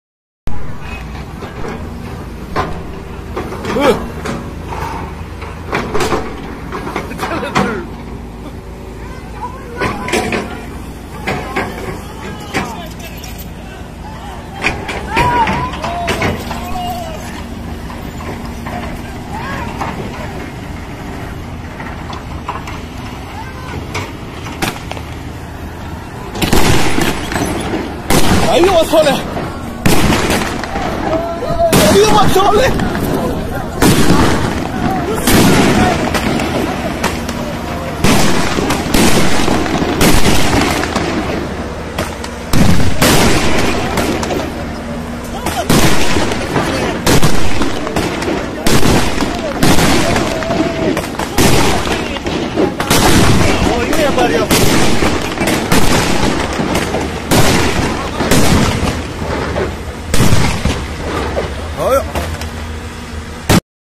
Chinese People Make Rockets to Shoot at Developers
Translated text: The Chinese are angry. Very angry. So angry that they take all the love a demolition crew attacked with home-made fireworks weapons. We do not have the official story, but expect from these people are put out of their homes. In China, land never really your property. You buy the rights for 72 years, while the government's right to hold marbles you at any time from your house. For the Beijing Olympics, for example, there are over one million people unceremoniously put out of their homes, but these Chinese are not easily afserveren.